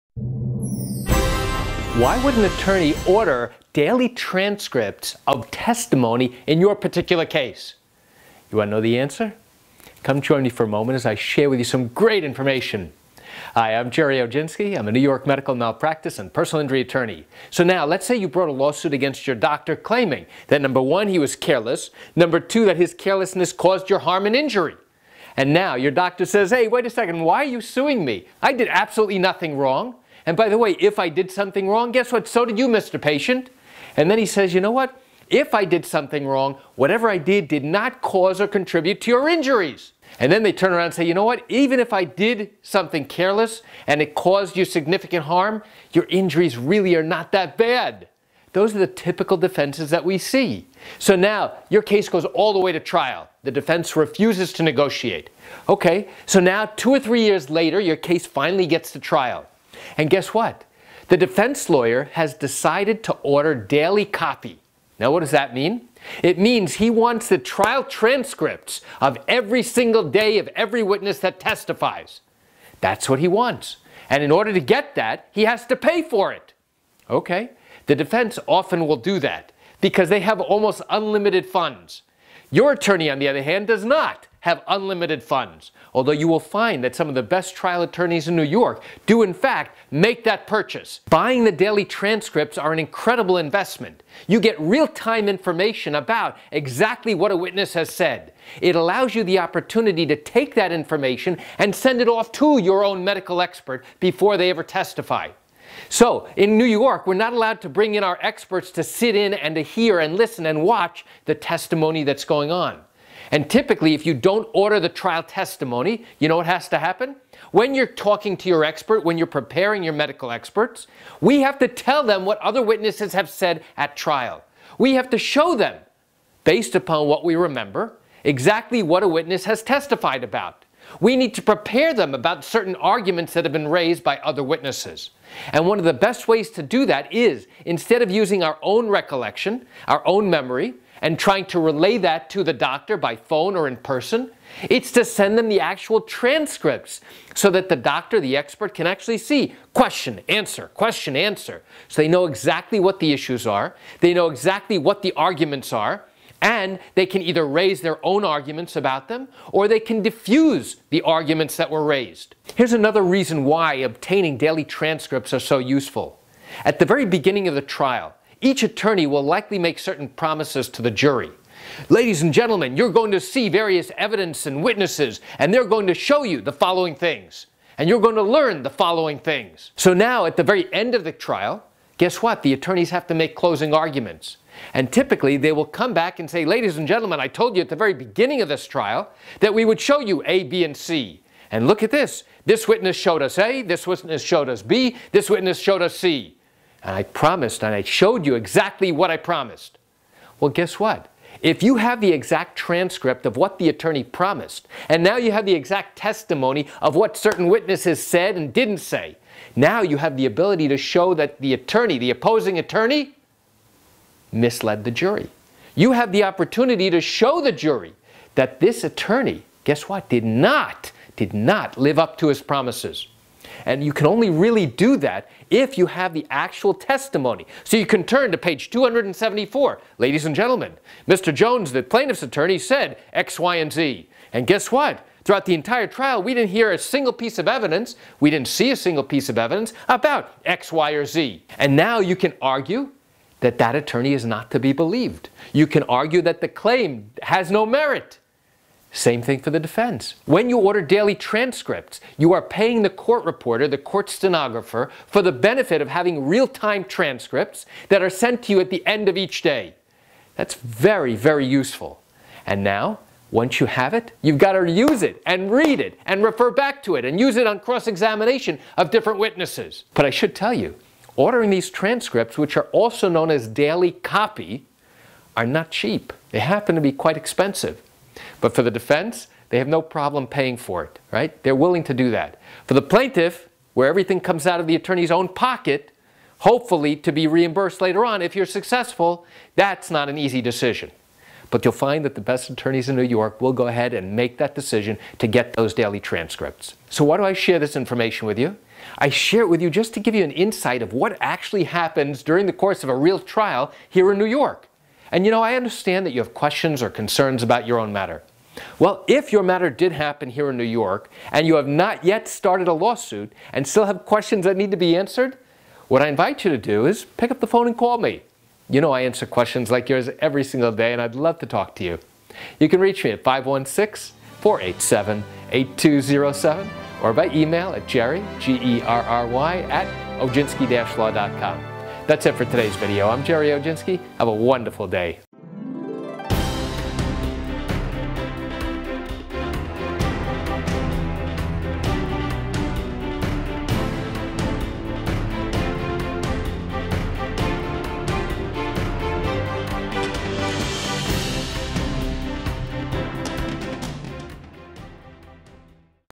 Why Would an Attorney Order Daily Transcripts in Your Medical Malpractice Trial Here in New York?
Why Would an Attorney Order Daily Transcripts in Your Medical Malpractice Trial in New York?
NY Medical Malpractice & Personal Injury Lawyer Gerry Oginski Explains

When your case gets to trial, there will be a court stenographer who records everything that is said in the courtroom. She will record every word the judge says. She will record everything your attorney says. She will write down everything the opposing attorney says.

Importantly, she will record everything the witness says.
Then, at the end of the day, she will transcribe her notes.
Those transcribed pages represent the trial transcript.

Each attorney has the opportunity to buy the trial transcript.

You can also order a real-time and uncorrected version of exactly what is said via your computer. The problem is that buying daily transcripts is expensive.

For the court reporter, to create daily transcripts is time-intensive.
That explains why it's costly to do.
However, you'll notice that defense lawyers in medical malpractice cases often will purchase the daily transcripts, also known as daily copy.

Some of the best plaintiff attorneys will also invest in the transcripts or at least portions of it.

Why would a lawyer buy the trial transcript or portions of it?

Simple. To know exactly what was said during opening arguments.
To know exactly what a witness said.
To be able to send a copy of the transcript to their own expert who will be testifying in court shortly.

To use during cross-examination.
To use during closing arguments.
If you can show exactly what a witness said or didn't say, it's is a powerful tool.

Here's a cardiac malpractice case where I was able to achieve a $6 million dollar settlement for my client

To learn more about how accident & medical malpractice cases work in the state of New York, I encourage you to explore my educational